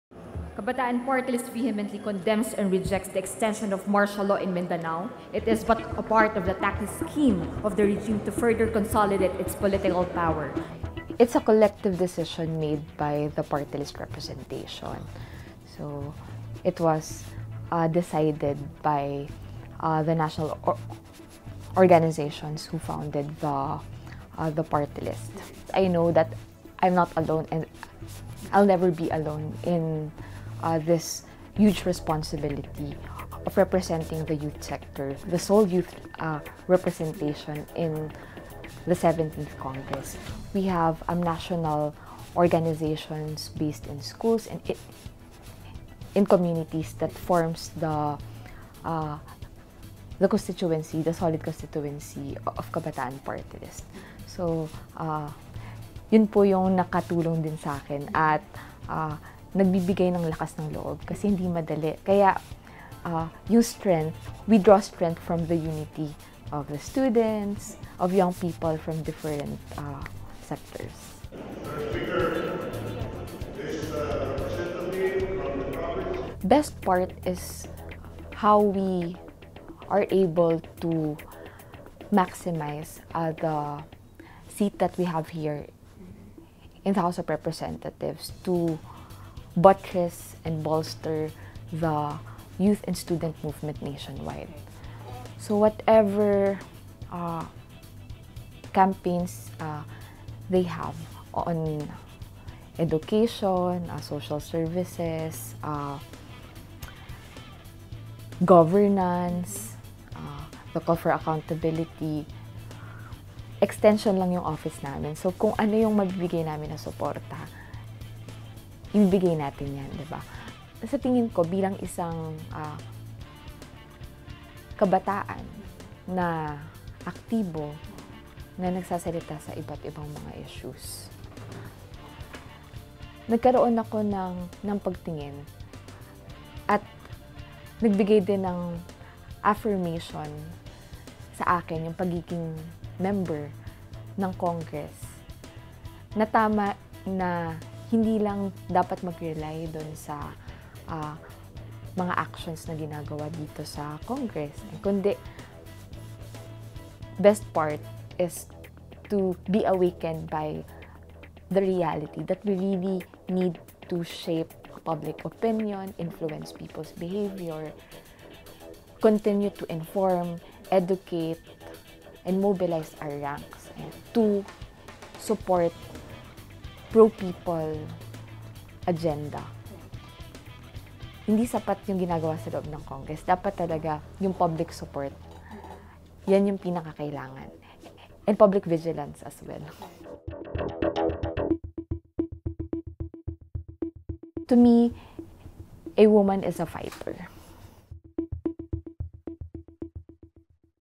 Sarah Elago on why being young and being a dissenter matters
More videos on Rappler Follow Rappler for the latest news in the Philippines and across the globe. “I’m still a full-time activist who just happens to be a congresswoman,” the 27-year-old Sarah Elago tells Rappler